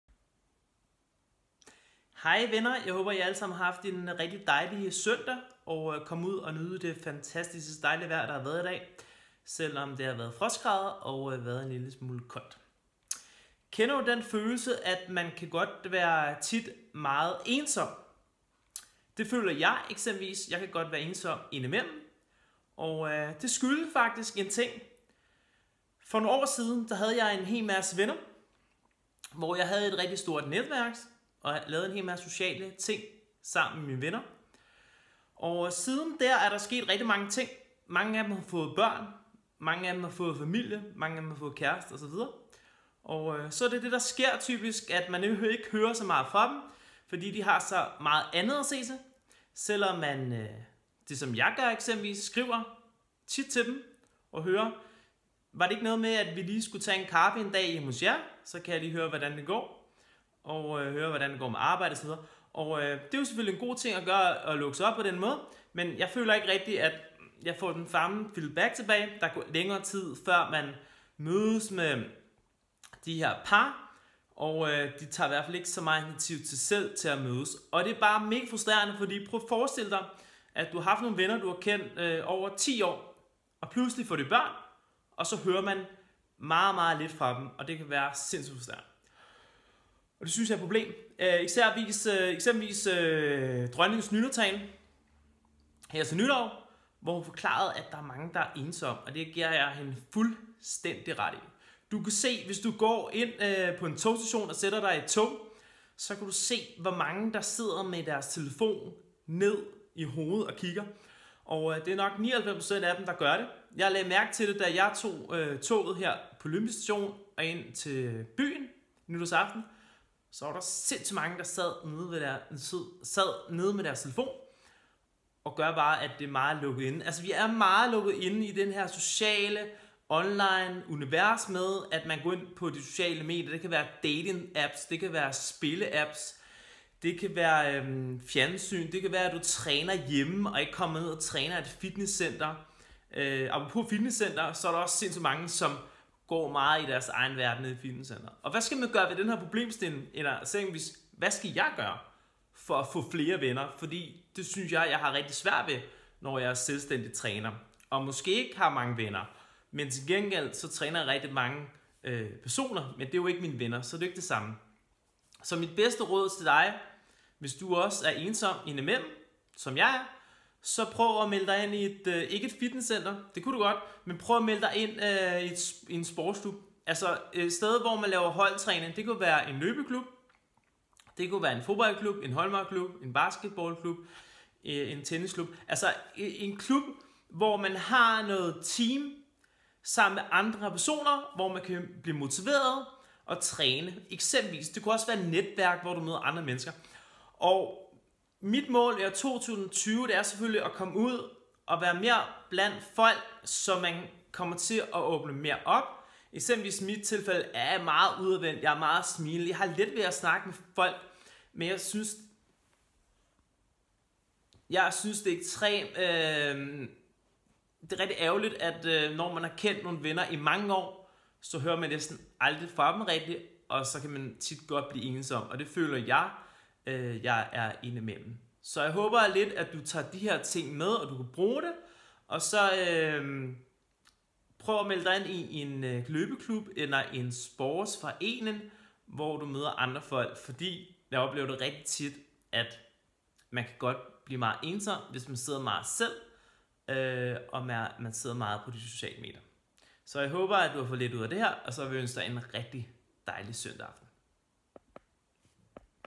Føler du dig indimellem ensom?
Kender du til følelsen, at du havde et stort netværk og en masse gode venner, men i dag har de fleste fået børn eller kæreste, så du ser dem mindre og høre meget let fra dem? Jeg kender godt til følelsen. Ja selvom du har kendt flere personer igennem flere år og de har været dine bedste venner. Høre du næsten aldrig fra dem. Så skal du høre min historie for det kunne være du kender til den samme følelsen.